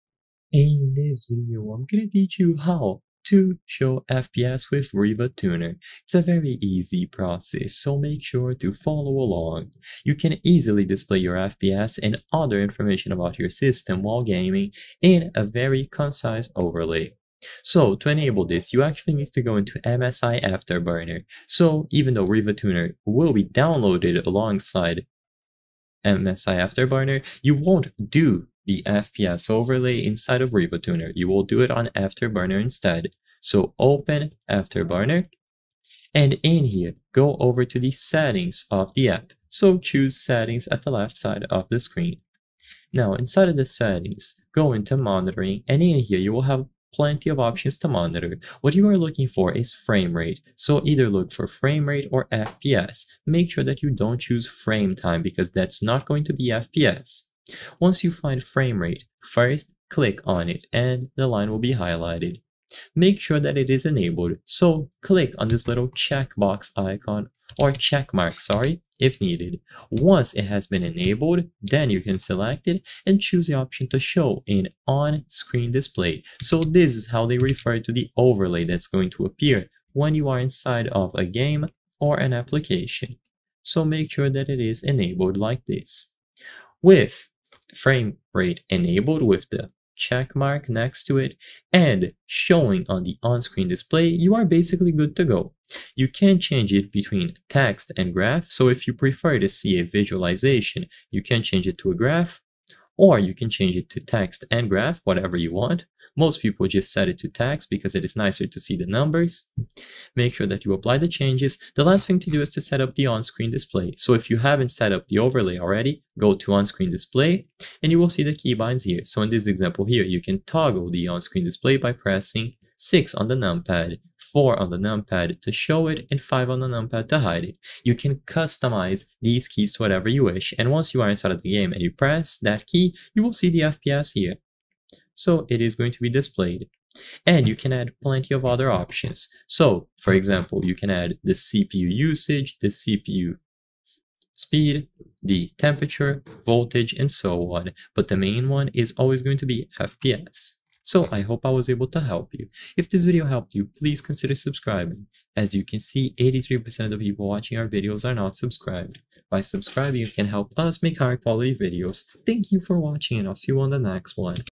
How To Show FPS With Rivatuner (2025) - Step by Step
I will solve your doubts about how to show fps with rivatuner, and whether or not it is possible to do this.

I hope I have helped you with the video!